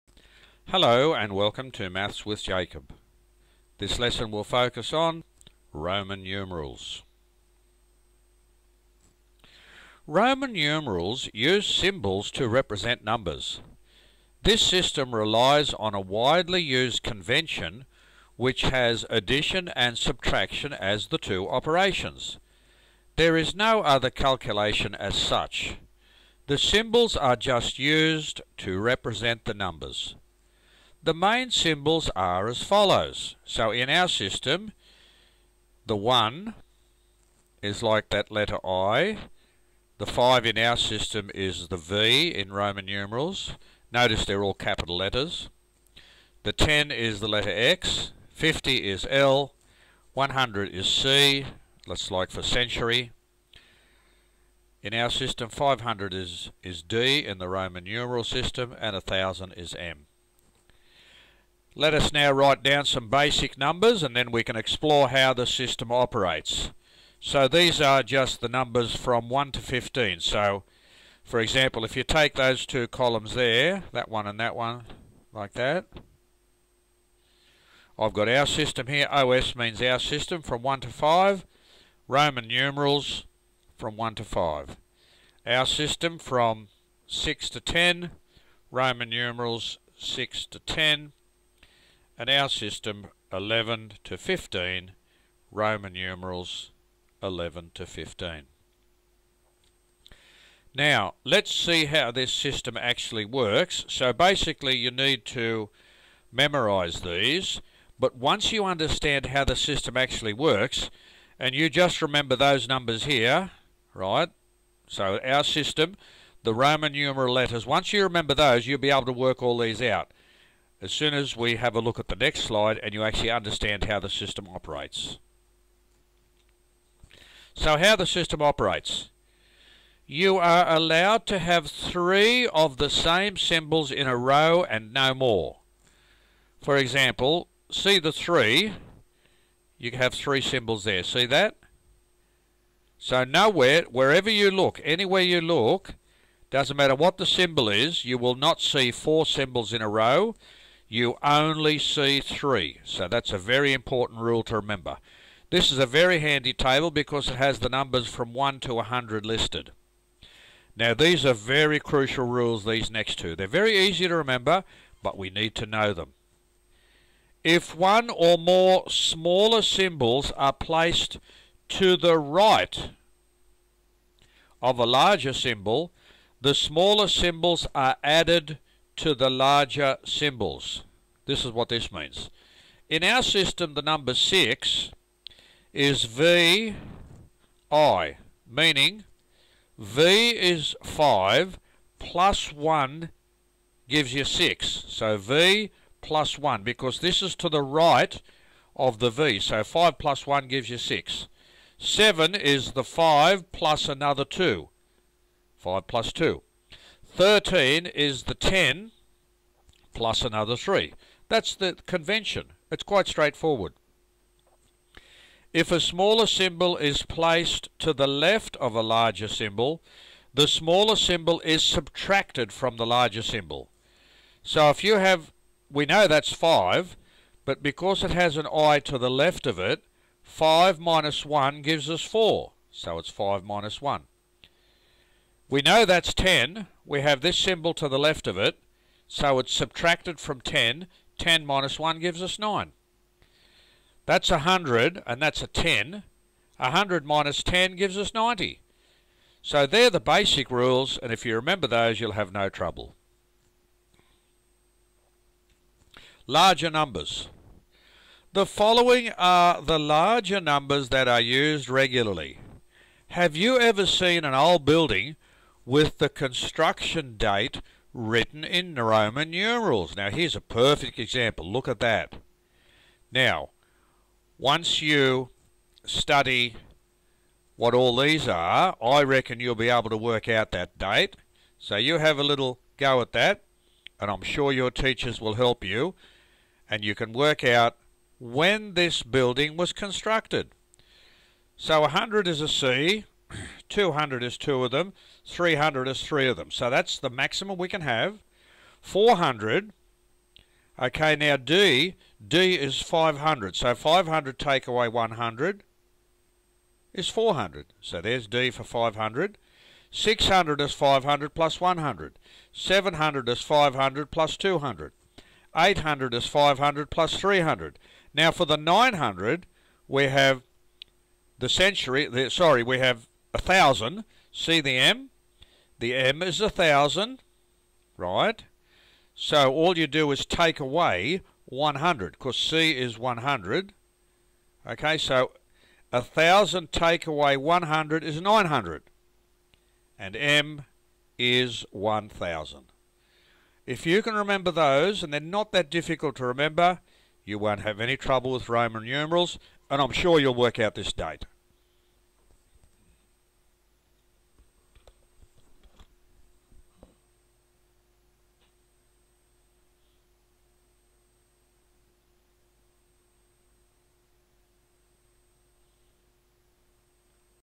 Roman Numerals
In this video we introduce the concept of Roman Numerals and we see the rules which govern this system.